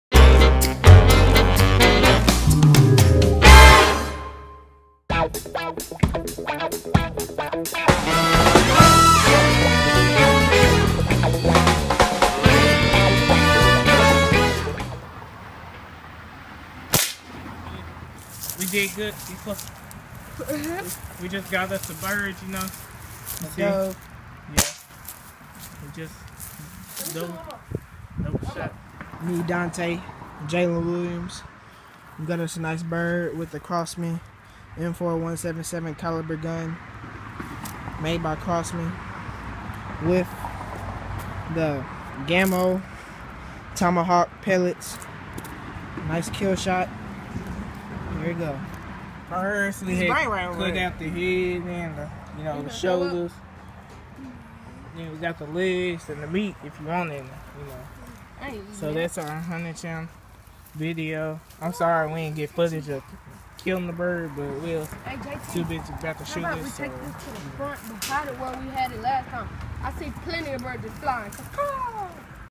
Bird hunting with crossman m4.177
In this video we went bird hunting with the crossman m4.177 pellet gun we got it in his wings but then had to put it out of his misery. Then we clean it and preserved the meat